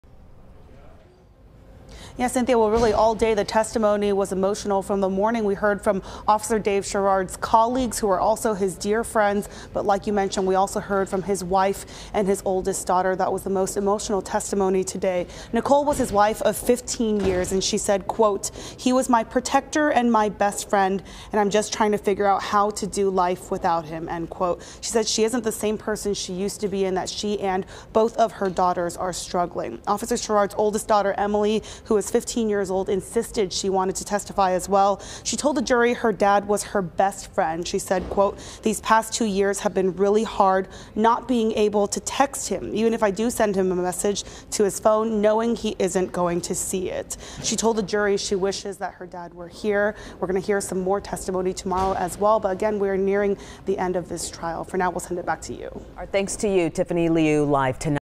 Family of slain Richardson officer testifies during sentencing trial of convicted killer
Emily Sherrard misses being able to text her police officer father throughout the day. She and her mother both took the witness stand to talk about David Sherrard